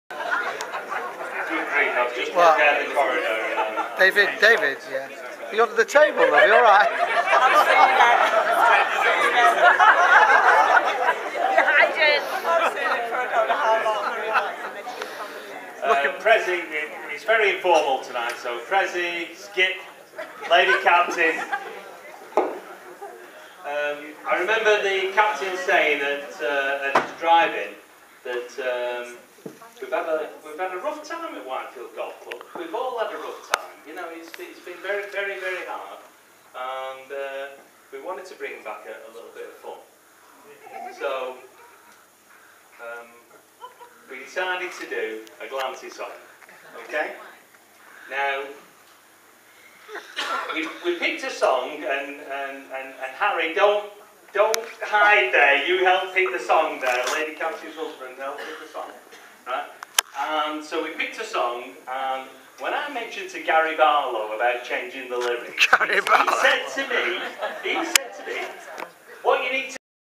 5 David Ballan Pt1
Whitefield Golf Club Captain's Glancy Dinner - David Ballan Part 1- City of Manchester Stadium - 1st July 2010